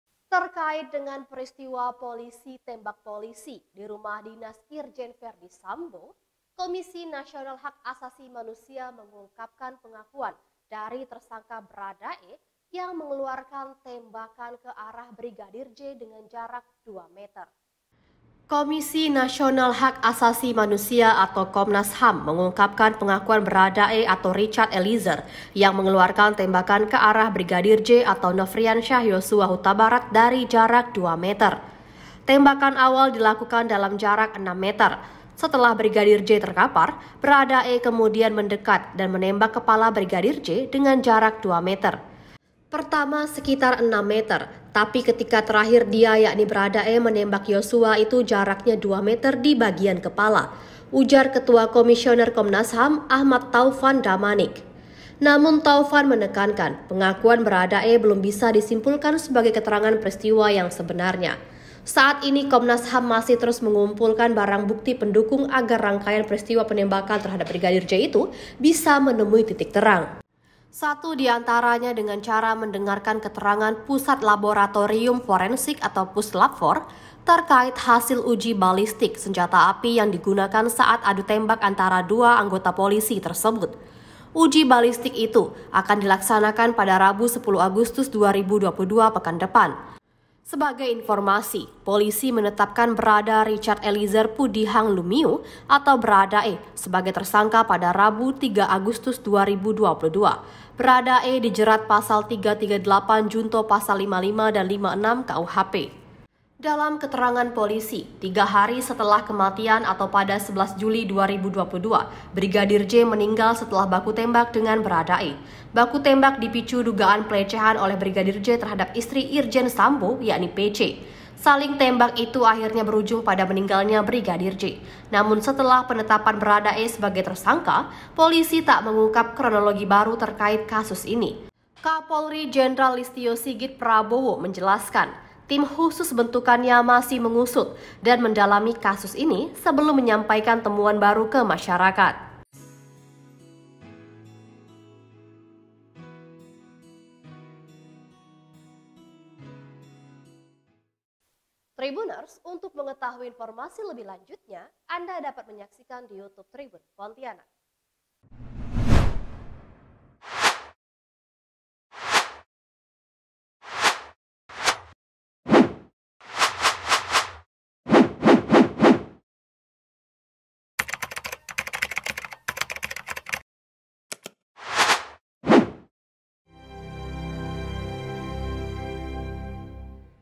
🔴 Bharada E Tembak Kepala Brigadir J dari Jarak 2 Meter, Komnas HAM Ungkap Pengakuan Bharada E
Komisi Nasional Hak Asasi Manusia (Komnas HAM) mengungkapkan pengakuan Bharada E atau Richard Eliezer yang mengelurkan tembakan ke arah Brigadir J atau Nofriansyah Yosua Hutabarat dari jarak dua meter Tembakan awal dilakukan dalam jarak enam meter.

Setelah Brigadir J terkapar, Bharada E kemudian mendekat dan menembak kepala Brigadir J dengan jarak dua meter.

Namun, Taufan menekankan pengakuan Bharada E belum bisa disimpulkan sebagai keterangan peristiwa yang sebenarnya.